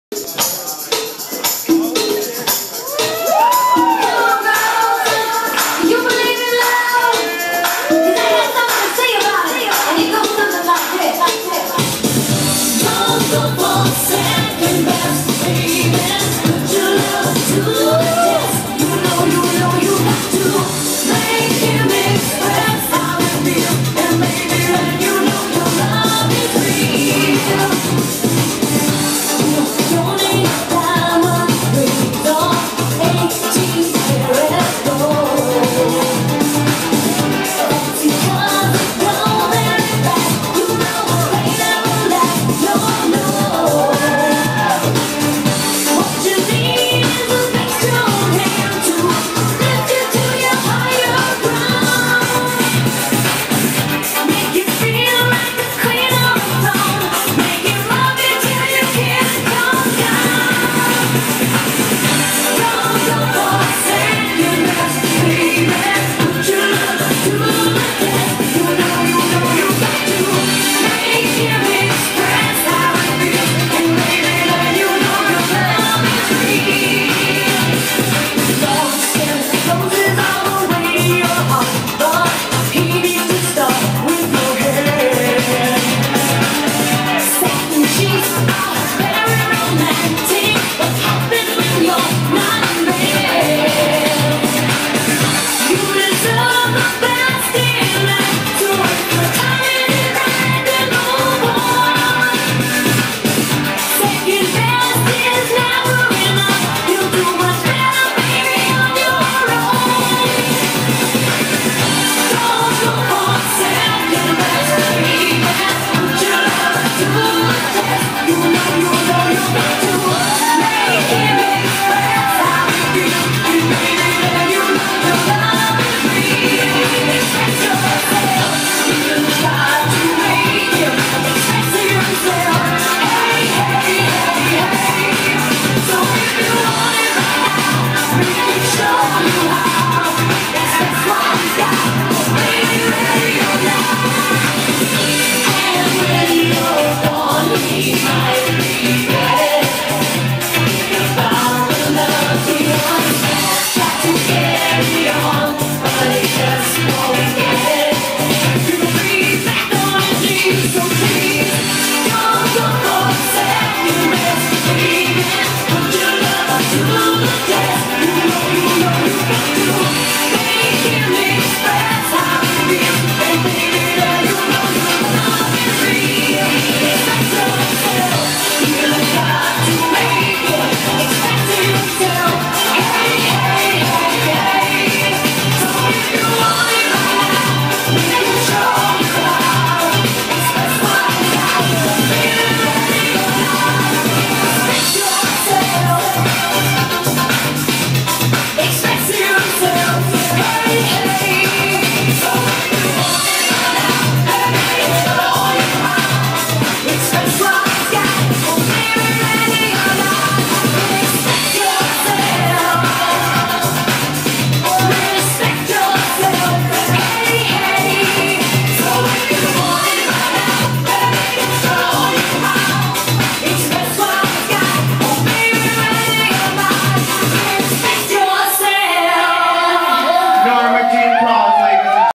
Norma Jean @ WHORE HAUS 12-12-12 Drag Reversal Night
Drag Reversal Night is a night where the other side of WHORE HAUS gets to do Drag.  From the DJ, photographer, performers boyfriend, and one of our Regulars.